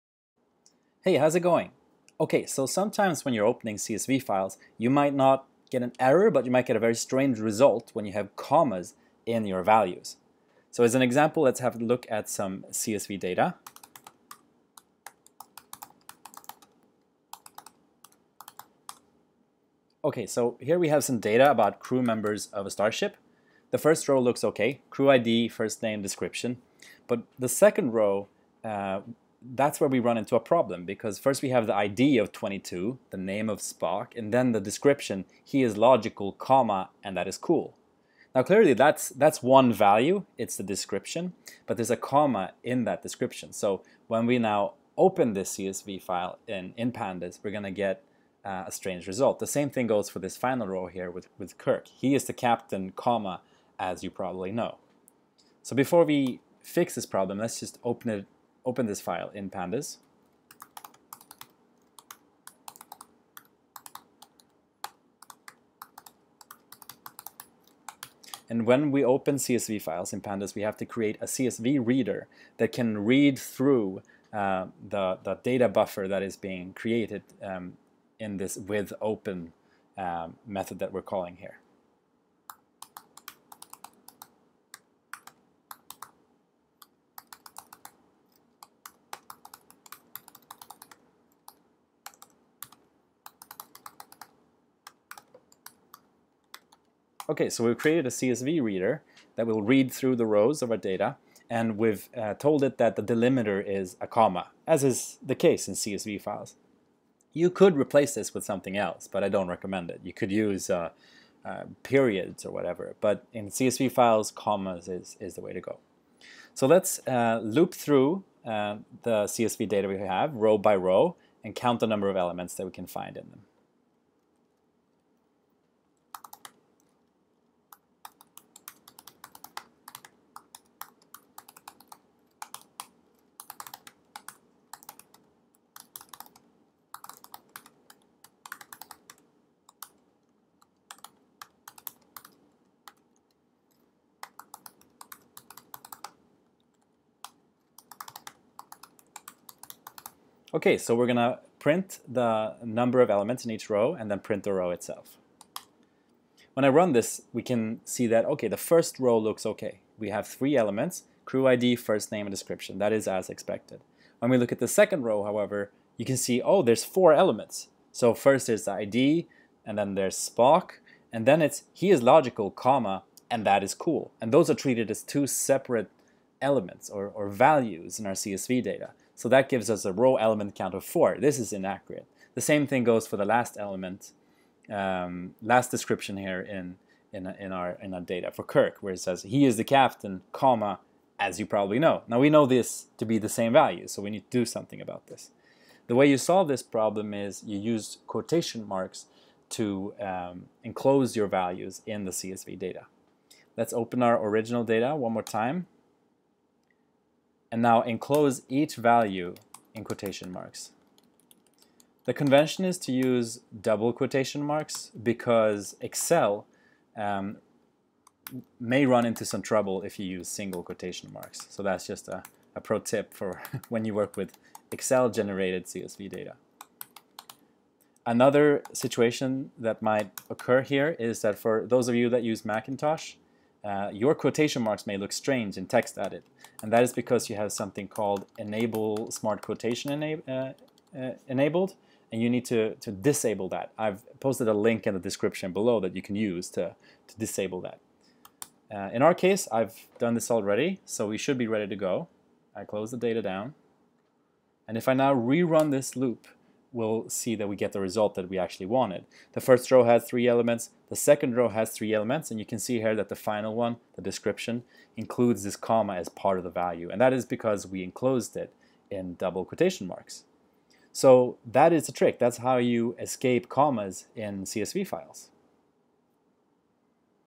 Escaping Commas in CSV Data
At some point, you will encounter Comma Separated Value (CSV) data that uses commas within its values. This is potentially a big problem because your values may end up being unintentionally separated. In this tutorial, I will walk you through how enclosing your values in double quotation marks can ensure that your values remain intact when you load them in Python using Jupyter.

Some of you may run into the problem of smart quotes being the default in your Text Editors. In Mac, you can disable this setting using the information found in this stack overflow thread